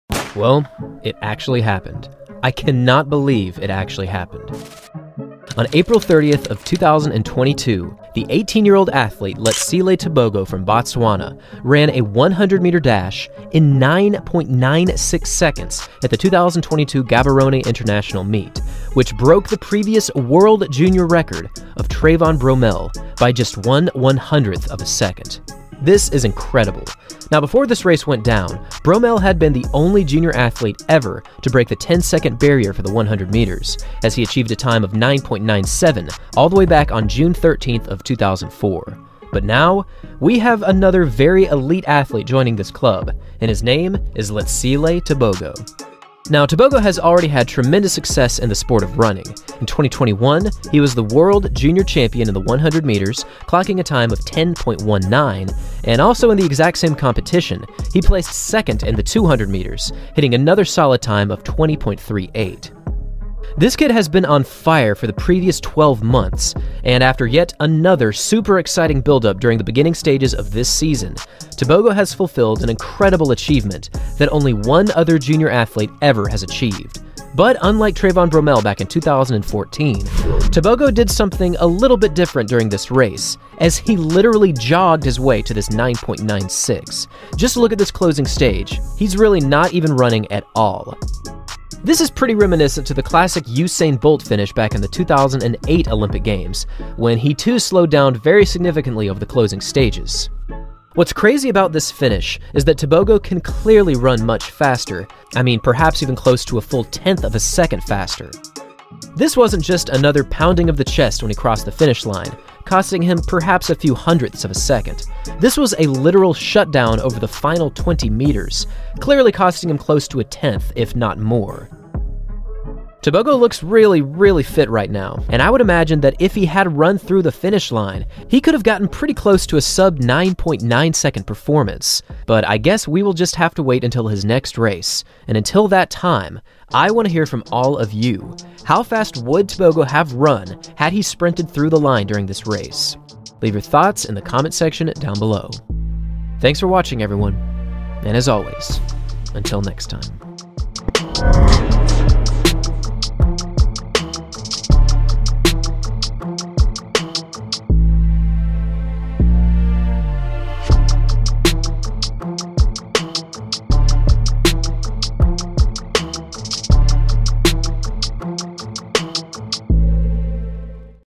I CANNOT BELIEVE It Actually Happened! || Letsile Tebogo Breaks The WORLD JUNIOR 100 Meter Record
Letsile Tebogo could be the next great sprinter